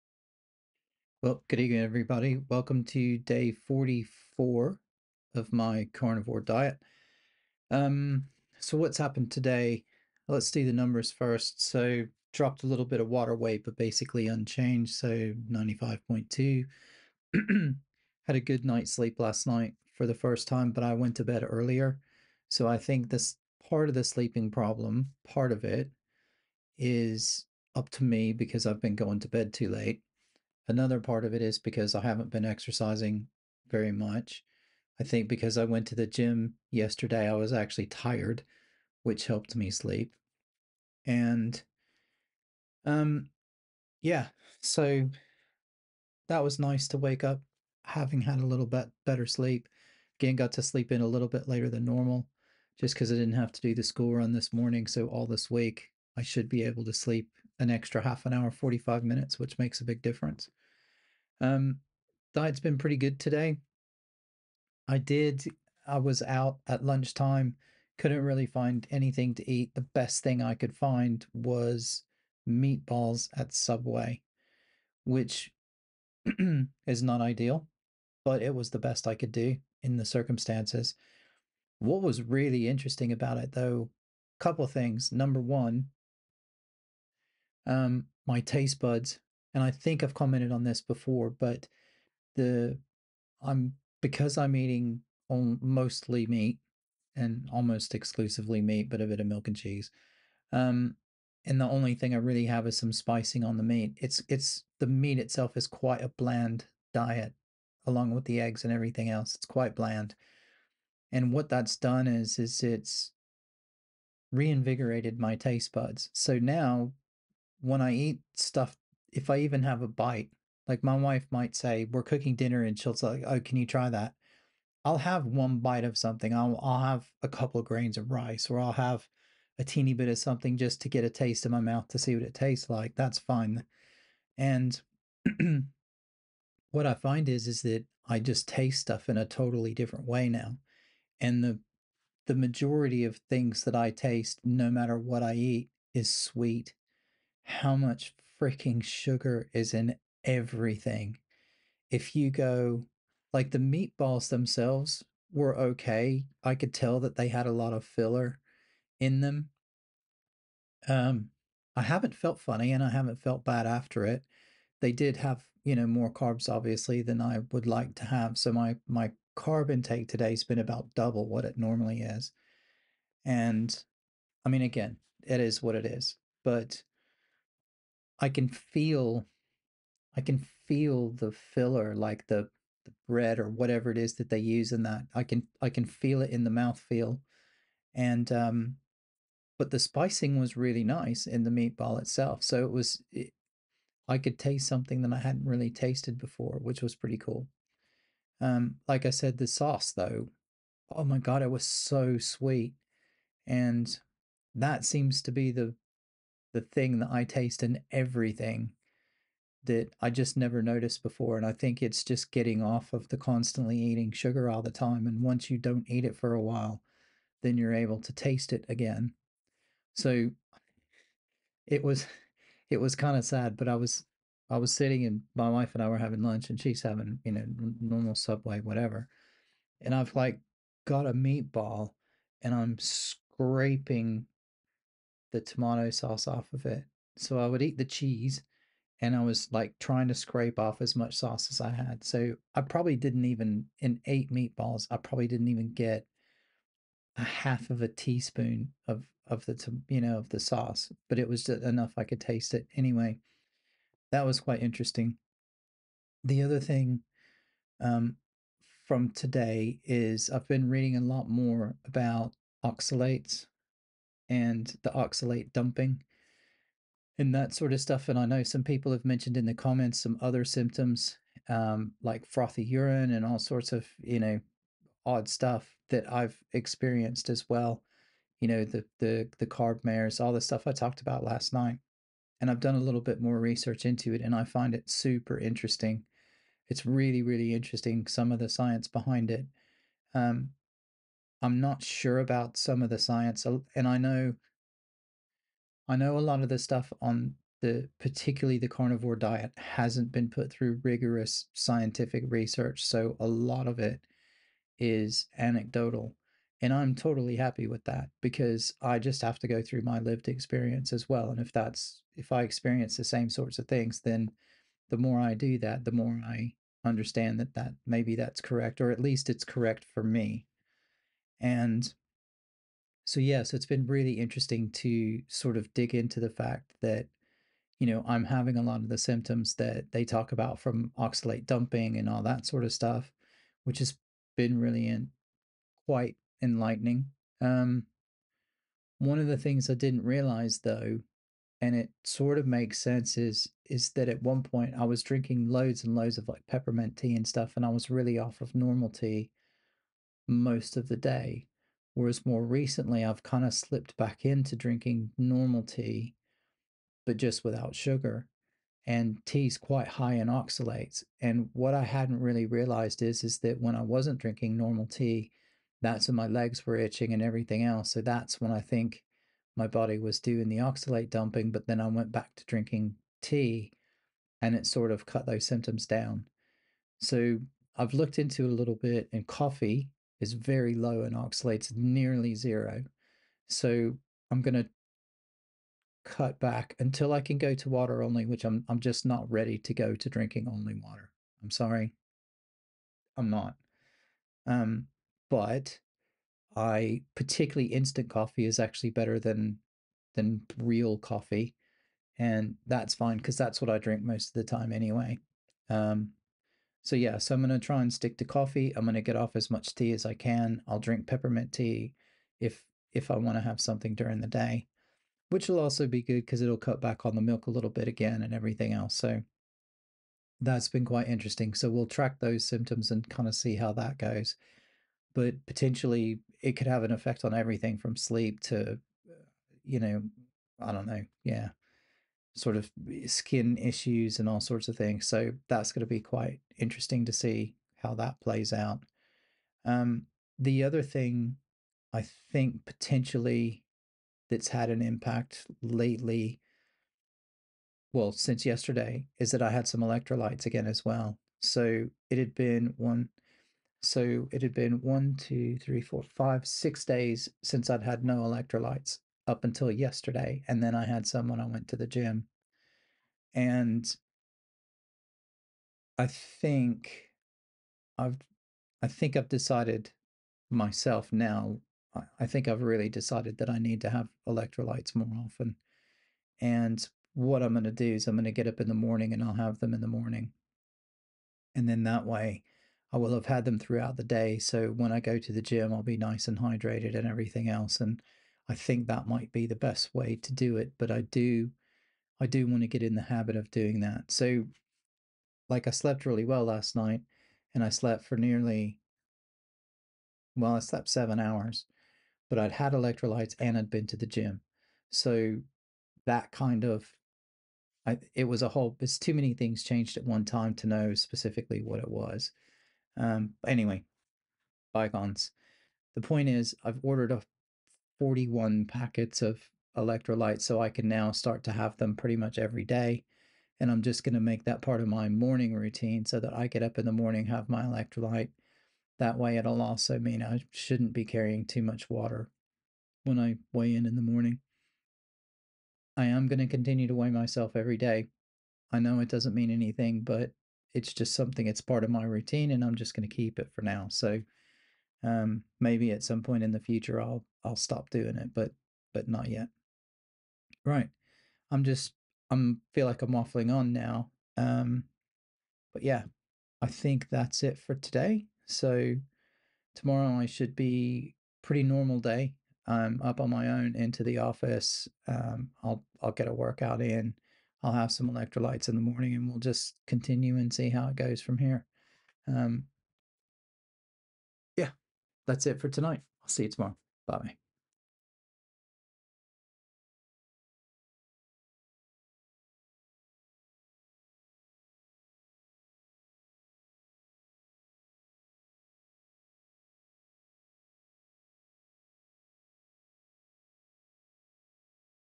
Carnivore Diet Day 44 - Falling down an Oxalate rabbit hole
It's been a pretty typical day today, but I've been going down an Oxalate rabbit hole, watching loads of videos about it - specifically, Oxalate dumping. As I mentioned yesterday, it's good to know that I'm not the only one experiencing some odd symptoms and that there seems to be an explanation for why they are happening. It also indicates to me that I'm on the right track.

**You may notice that the sound is different in this video; that's because I forgot to plug in my proper microphone, and it recorded everything on my laptop. Everything should be back to normal tomorrow.